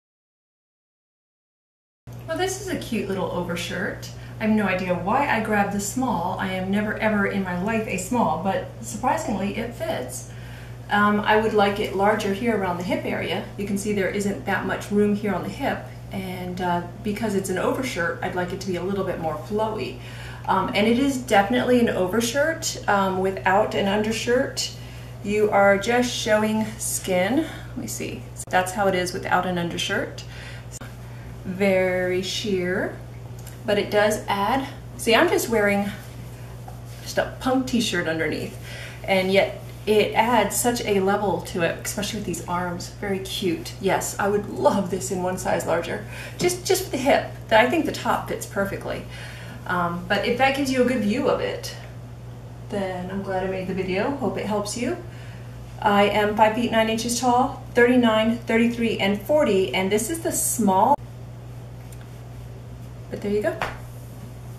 Aichatan Konater
As I say in the video, I never wear a small, so I feel kind of flattered that this shirt is already flattering me with the size tag.
And it flatters with the fit as well, but not exactly like it shows in the picture.  At least not for me.  Length wise, I may be taller than the model, which might explain why mine only falls to my hip and not the crotch as it does for her, but there is also the way she wears it up top.
If she is so much smaller than me that it would go five inches longer on her, then the top should look baggier.  But hers is pushed into position so that a deep V is formed and both sleeves stay on the shoulders.  If it is not maintained, the shirt is wide enough on top to go off shoulder.
I can’t tell what she has underneath the shirt to make it opaque, but I had to use a spaghetti strap t-shirt.  Just a junk shirt I do house work in.  But it certainly dressed that junky shirt up fast!
Anyway, I hope the video will help you determine the perfect size to grab for yourself.  If you are my size and you like the way it fits me, I am wearing the small.  I am 5’9”, 160 pounds.  40 inches in the bust and hips, and 33 in the waist (which is three fingers above the belly-button).